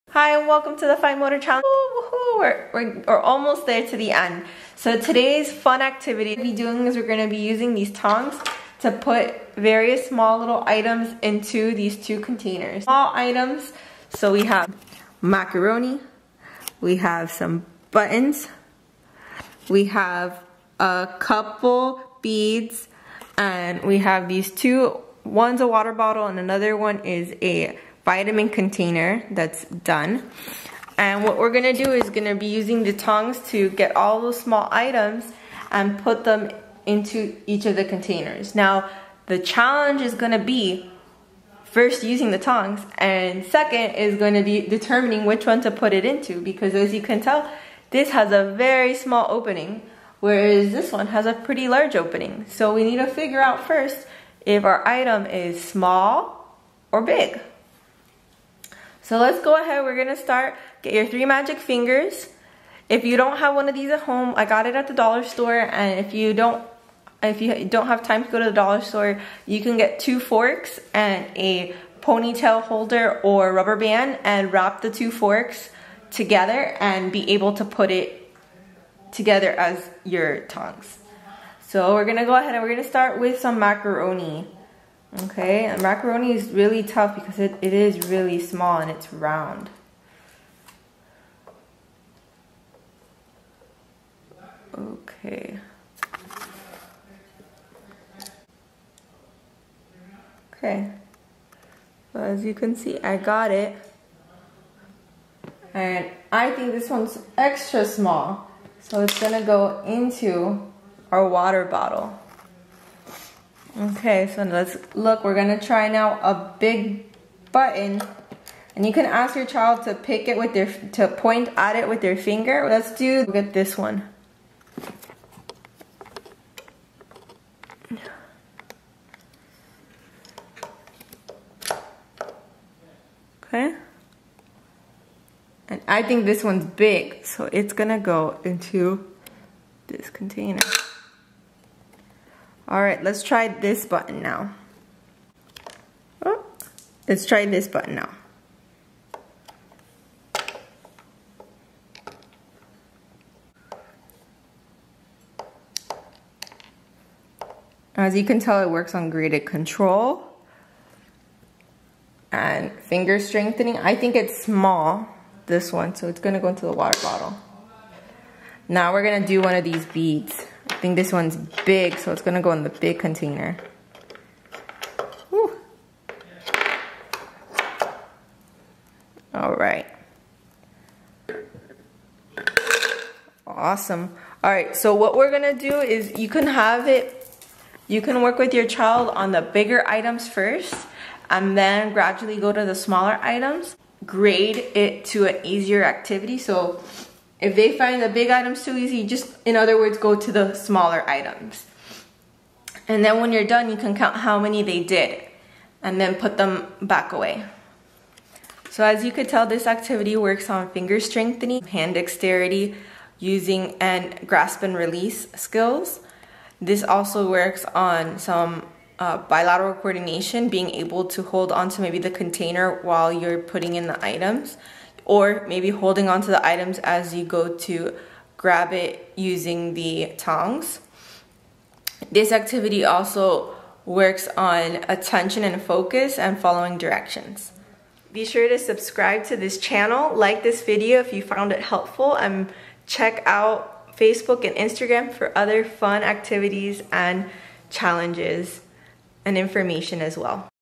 Small Item Clean Up: Fine Motor Challenge Day 21
This clean up activity works on hand dexterity, finger strength, grasp and release skills, tripod grasp, bilateral coordination,
motor planning, cognitive skill identifying small vs big.